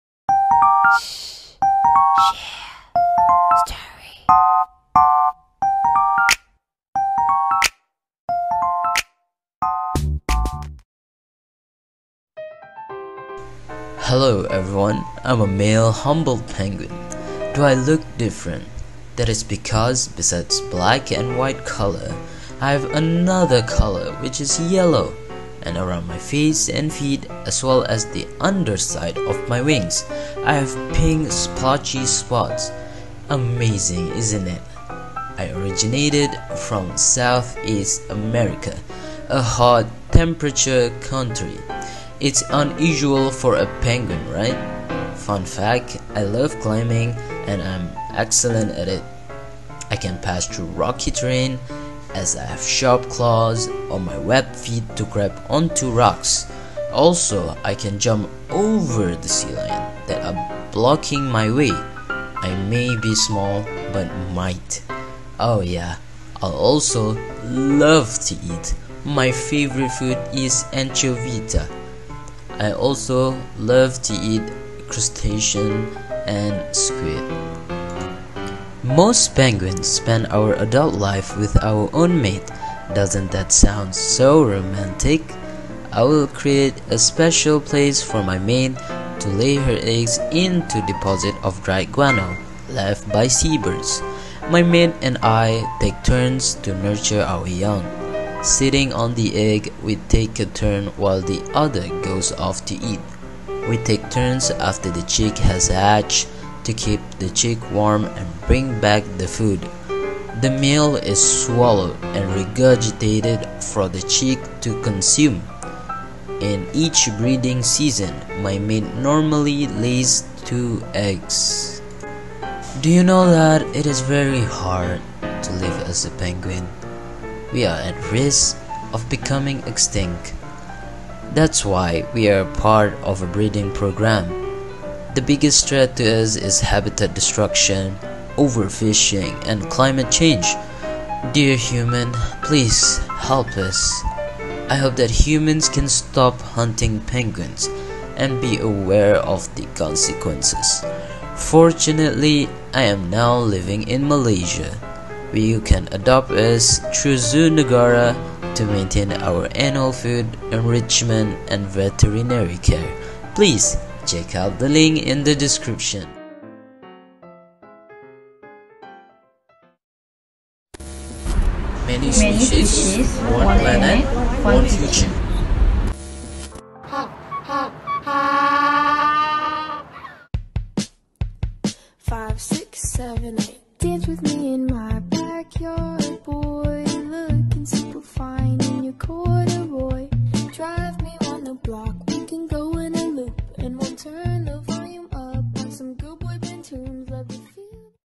10 001 Animals Action : Penguin 🐧
Together we can help the animals in Zoo Negara by adopting them

In collaboration with Zoo Negara, we are hosting a program called 10001 Animals Action where we have taken the initiative to raise awareness on the hardships of the animals living in Zoo Negara during these hard times.

In our videos, we will be telling you amazing stories and facts about these magnificent creatures while teaching you on how to make cute origami of them, 1 animal will be covered per video so look out for the other 6 videos!